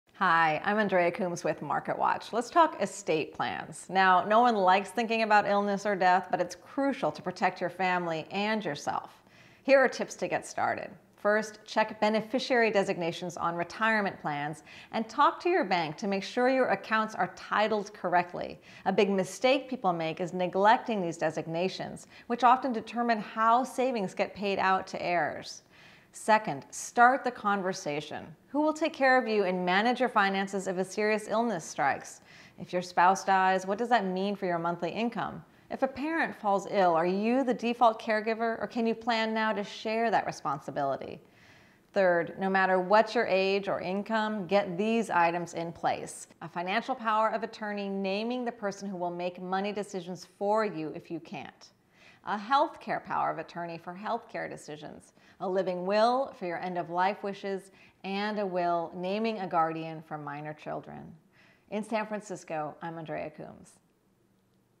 Do You Have An Estate Plan? You Need One
Nobody likes to think about illness or death, but planning is essential to protect your family and make sure your wishes are followed with regard to your assets and estate. Here are some easy, quick steps to get started.